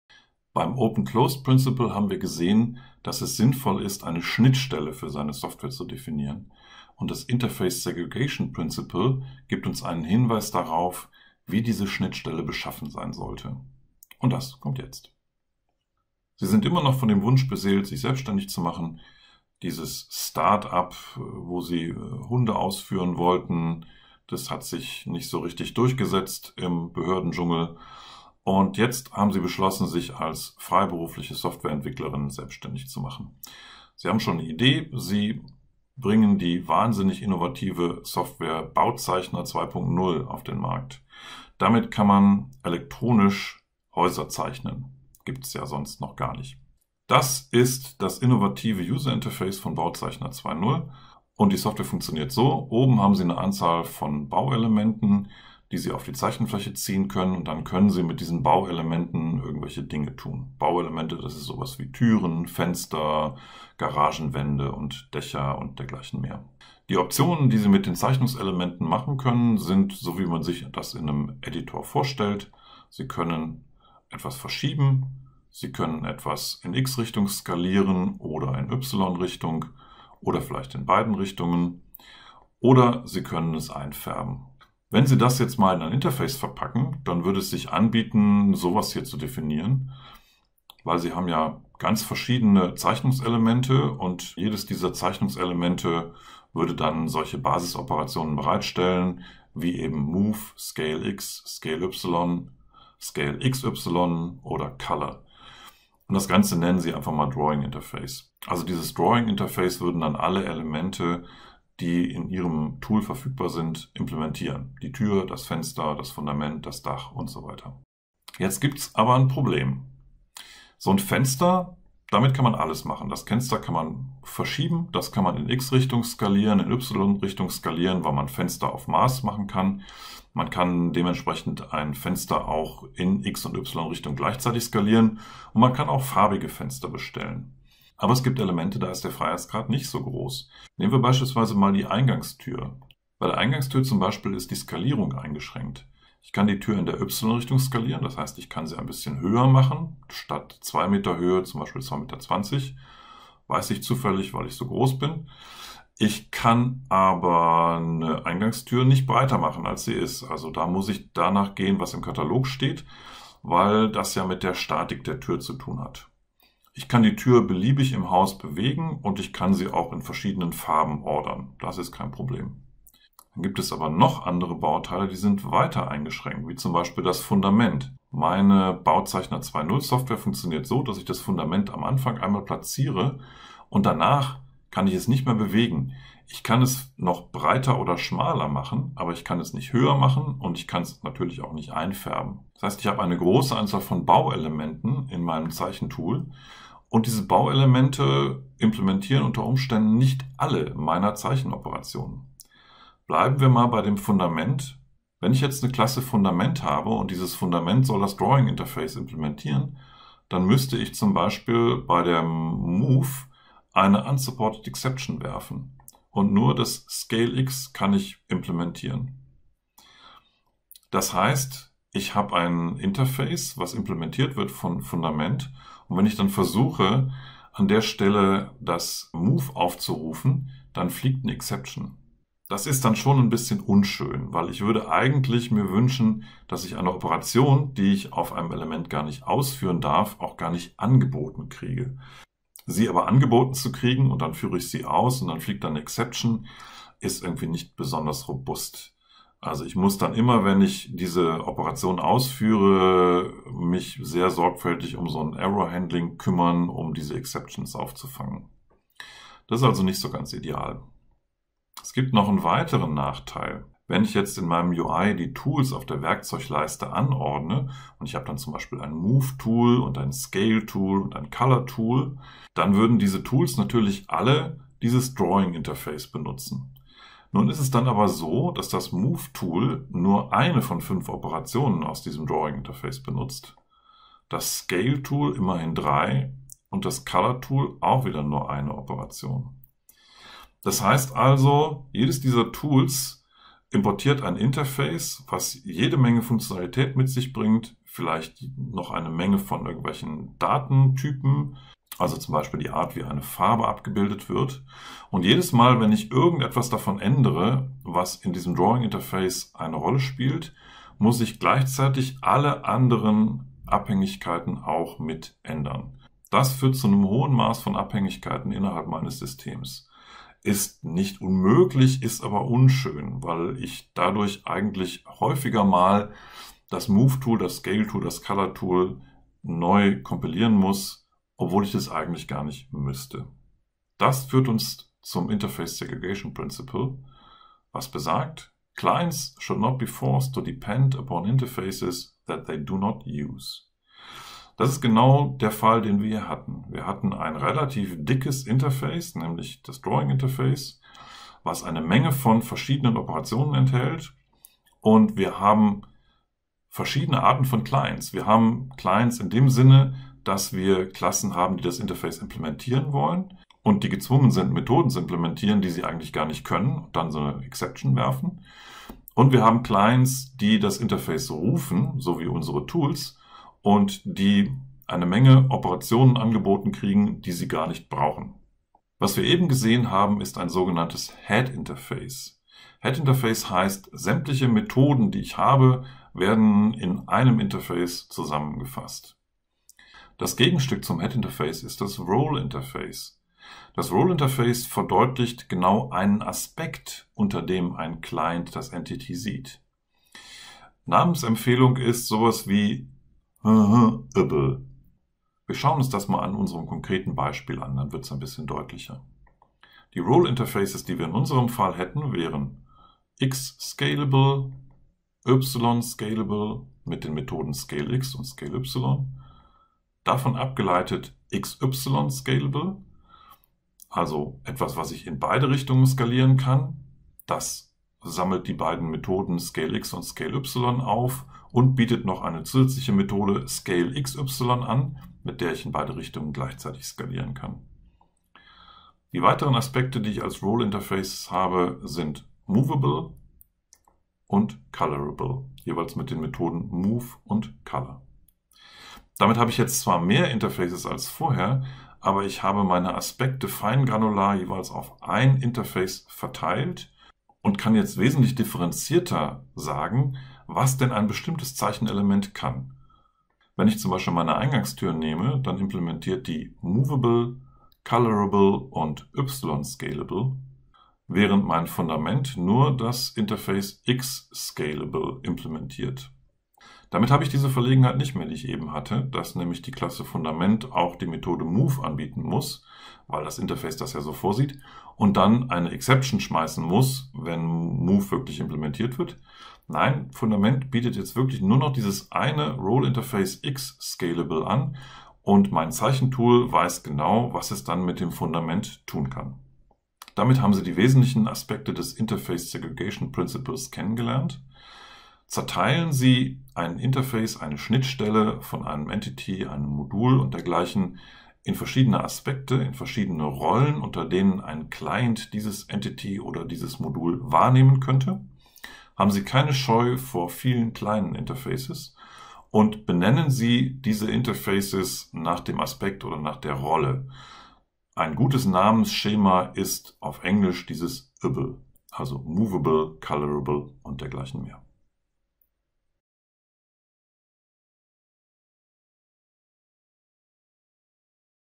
Interface Segregation Principle
Das Interface Segregation Principle besagt, dass Clients nur diejenige Funktionalität sehen sollen, die sie auch brauchen. Das reduziert Abhängigkeiten im System. In diesem Video erkläre ich das an einem einfachen Beispiel.